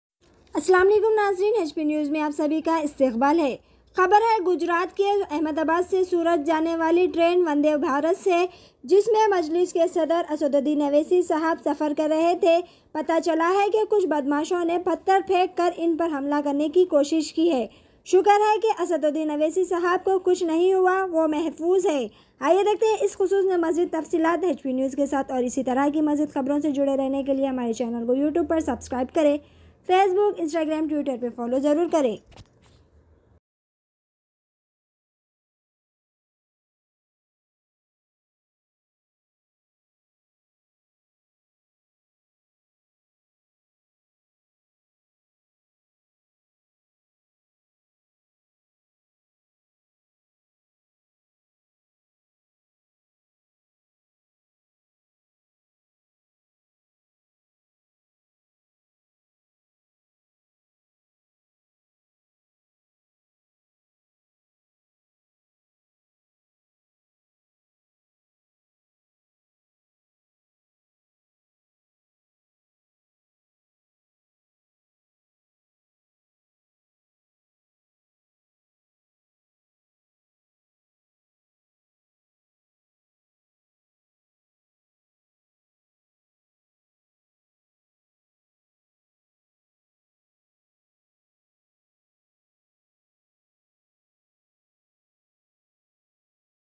🔴AIMIM Majlis Ke Sadar Asaduddin Owaisi sab per gujraat mai safar ke daoraan pathar phek kar humla?
Khabar Hai Gujraat Ke Ahmedabad Se Surat Jane Wali Train Vande Bharat Se Jismai Majlis Ke Sadar Asaduddin Owaisi Safar Karrahe The Pata Chala Hai Ke Kuch Badmashoo Ne Pathar Phek Kar Hamla Karne Ki Koshish Kiya Shukar Hai Asad Sab Ko Kuch Nahi Huwa Woo Safe Hai. hp news channel